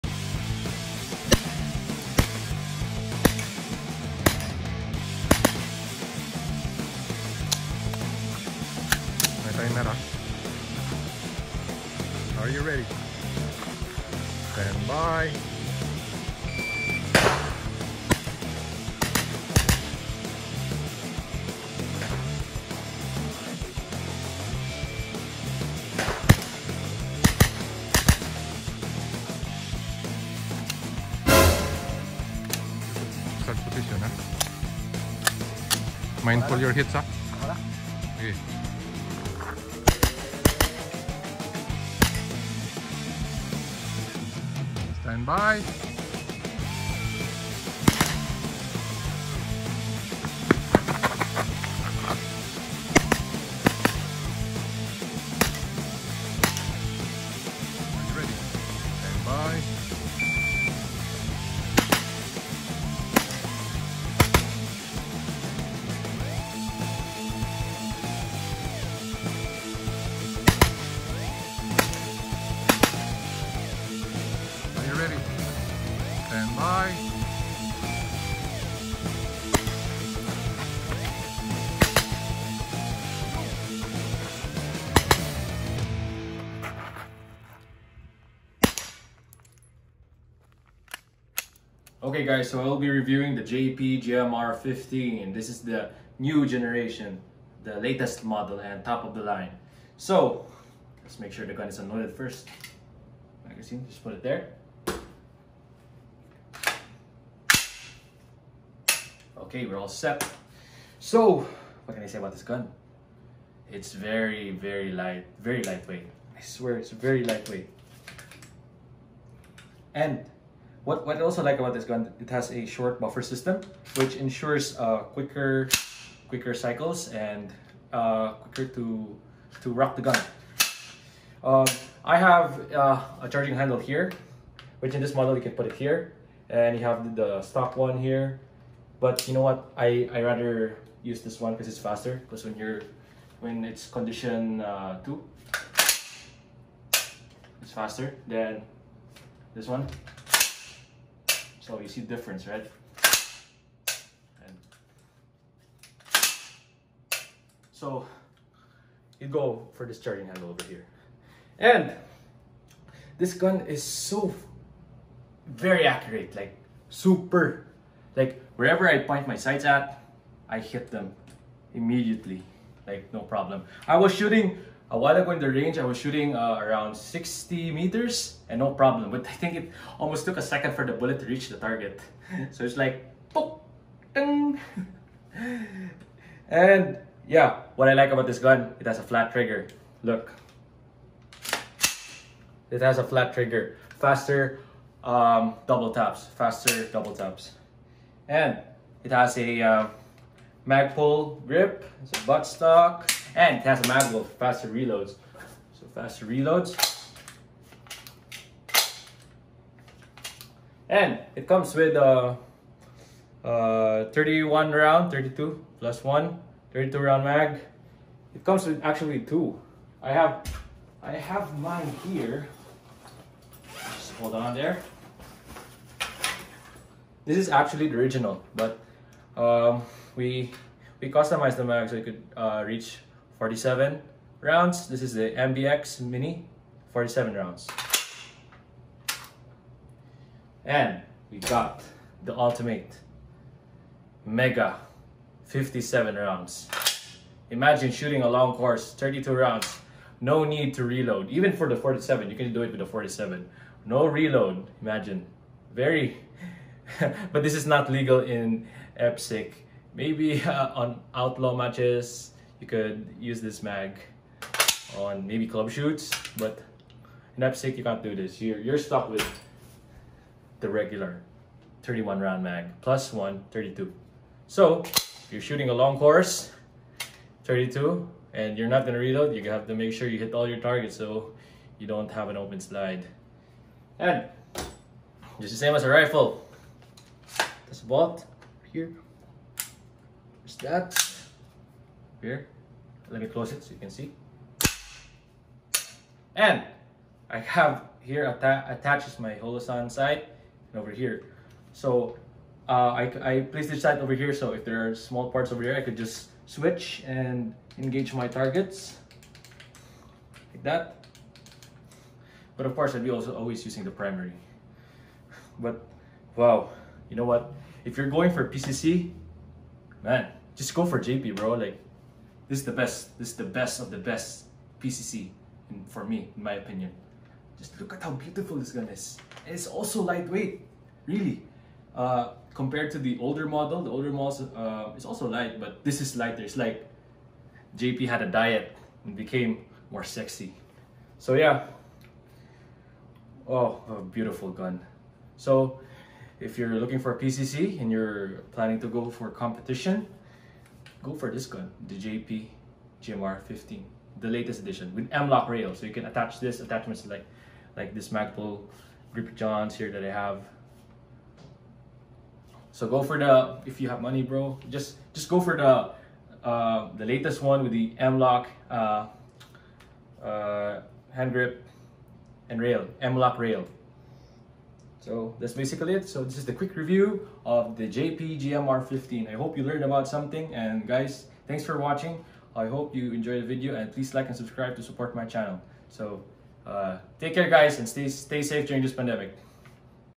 Next Gen JP GMR-15 9mm PCC (Quick Review)
The latest Model of the JP GMR-15 9mm Pistol Caliber Carbine it is lighter due to the 13.8" light contour barrel with Titanium comp, new light m-lok handguard, short stroke silent captured spring buffer system for faster cycling, flat match grade trigger with light trigger pull and short reset, "fattie" magwell, JP Raptor ambi charging handle, competition style side charging handle. Stay Safe!